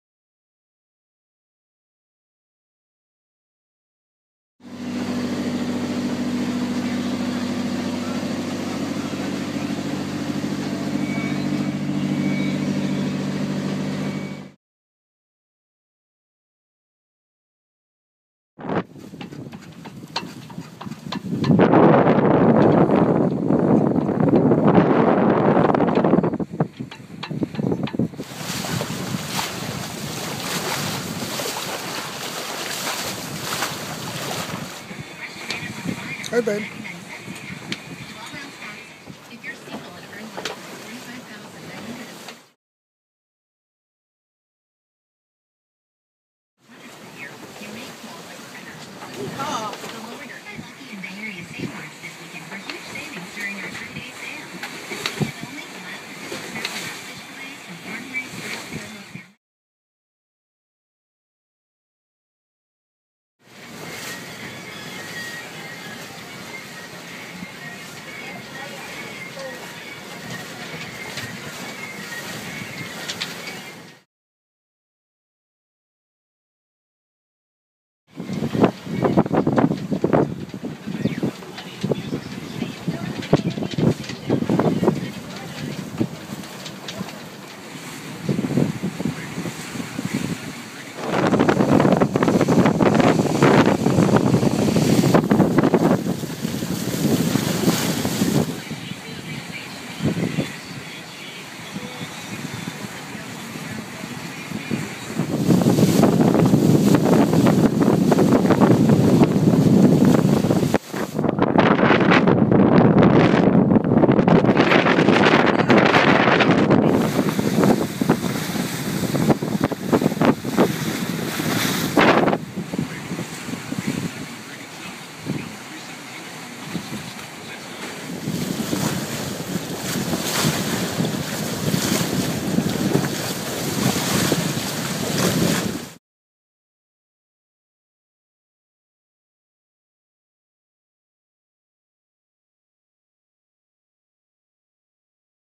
Iko Iko Video Sail Log 3/22/14
Iko Iko is a Classic Cross 36' trimaran design in San Francisco Bay. From San Leandro to Coyote Pt & back. It started out a 14 knot breeze then it piped up to 19 with gust to 23. With a single reefed main, 100% genoa and full mizzen we consistently sailed between 7-12 knots in sharp choppy wind waves.